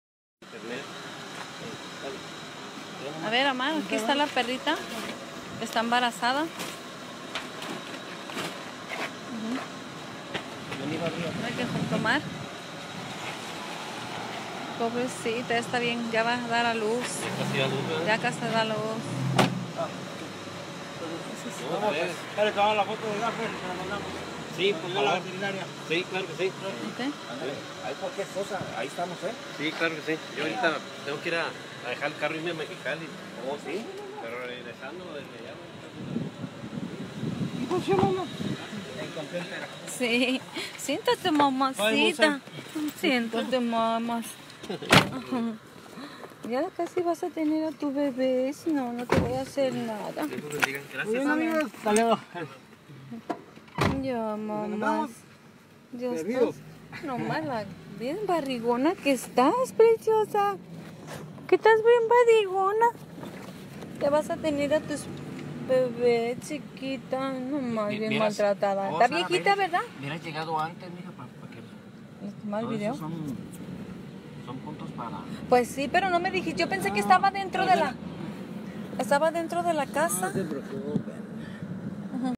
Una mama mas rescatada
Transportando a una mama en equipo